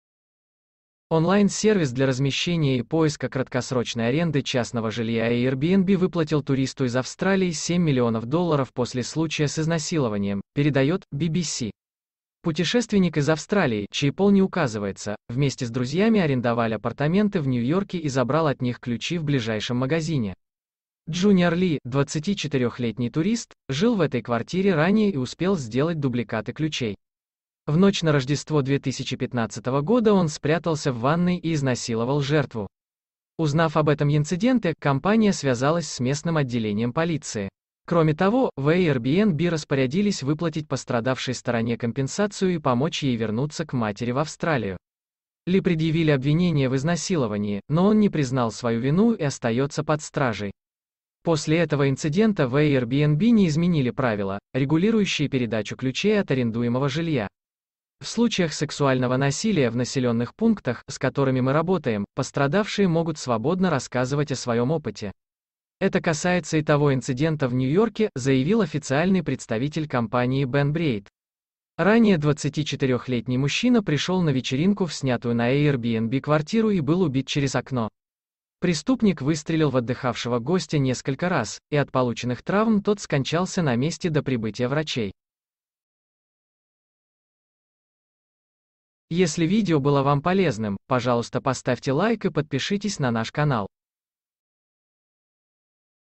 Airbnb выплатил туристу семь миллионов долларов после случая с изнасилованием
Онлайн-сервис для размещения и поиска краткосрочной аренды частного жилья Airbnb выплатил туристу из Австралии семь миллионов долларов после случая с изнасилованием. Однако после этого случая в Airbnb не изменили правила, регулирующие передачу ключей от арендуемого жилья.